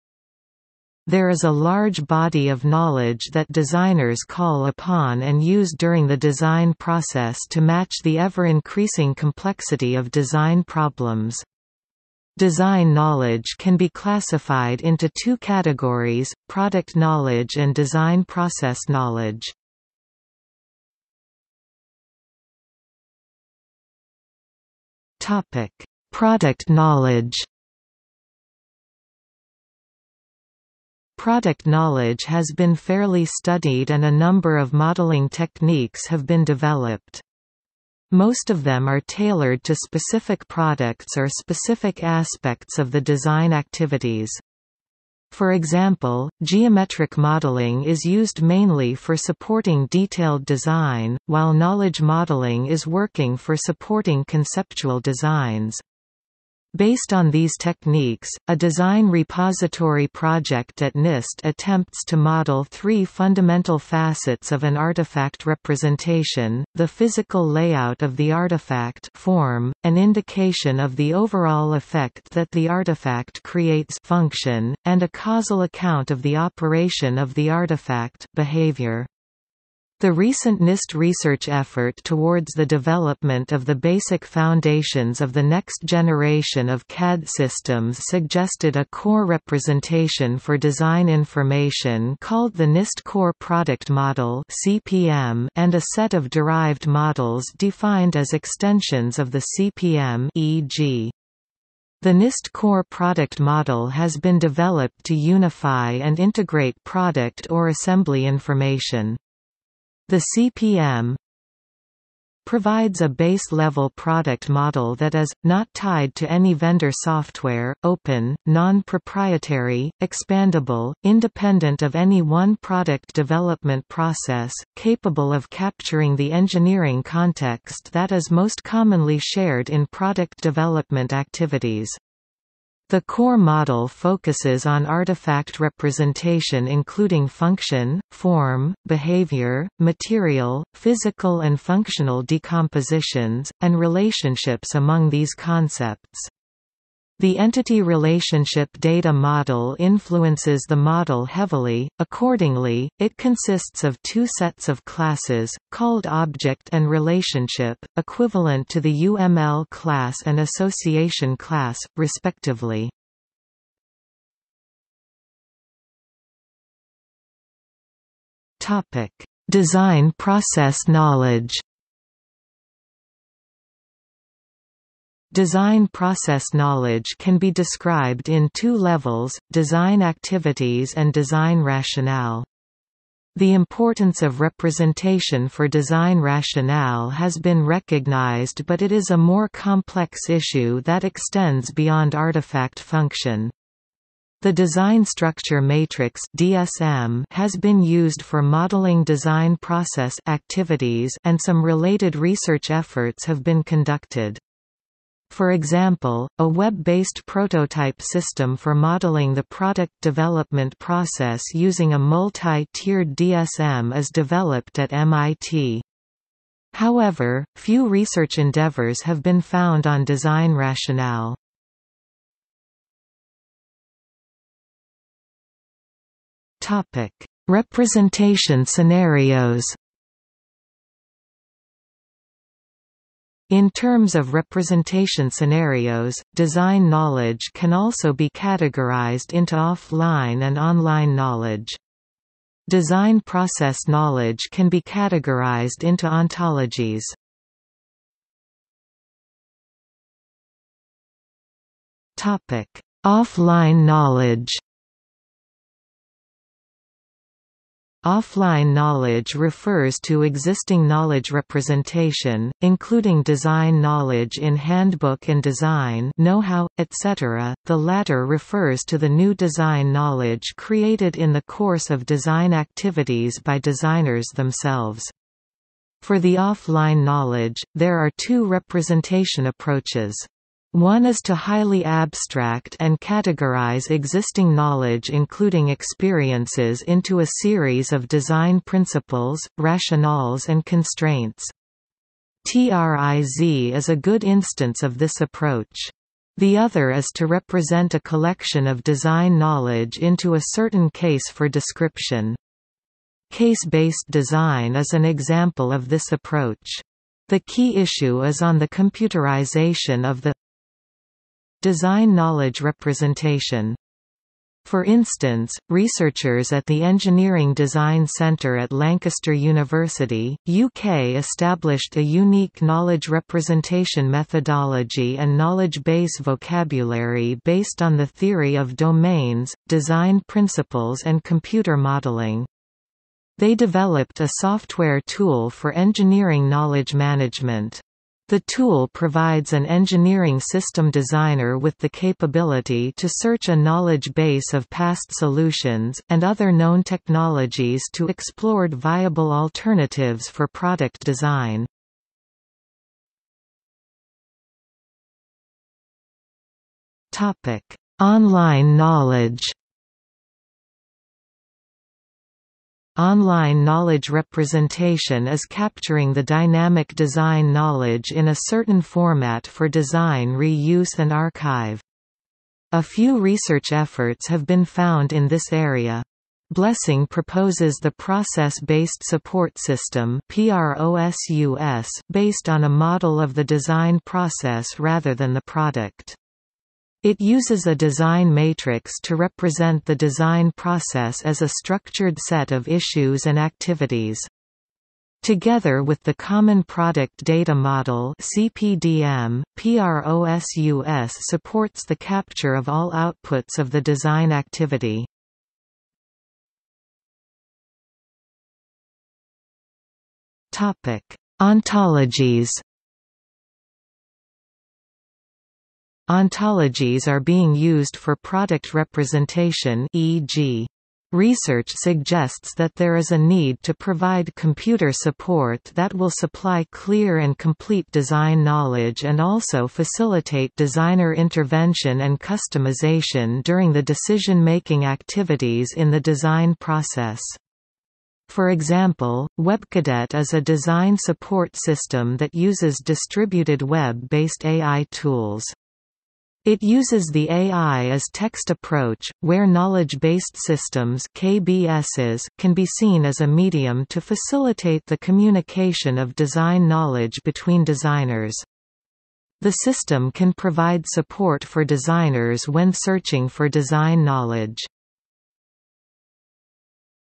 Design knowledge | Wikipedia audio article
00:00:21 1 Product Knowledge
00:02:45 2 Design Process Knowledge
00:03:43 3 Representation Scenarios
00:04:08 3.1 Off-line Knowledge
00:05:53 3.2 On-line Knowledge
00:06:47 3.3 Ontologies

- Socrates

SUMMARY
There is a large body of knowledge that designers call upon and use during the design process to match the ever-increasing complexity of design problems. Design knowledge can be classified into two categories: product knowledge and design process knowledge.